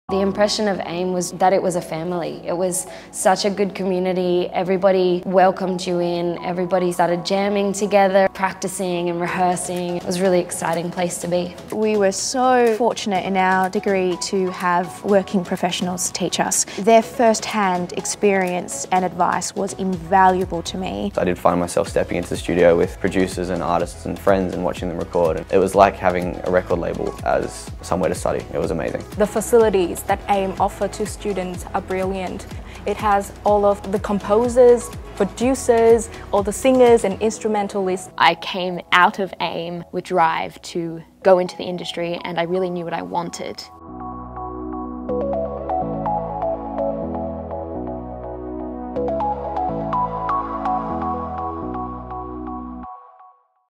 AIM Open Day 2019
Do you have what it takes to make it in the music industry?

Find out at AIM Open Day 2019 with live masterclasses, workshops and Q&As from industry pros.

Click the link now to reserve your complimentary entry - don't miss out.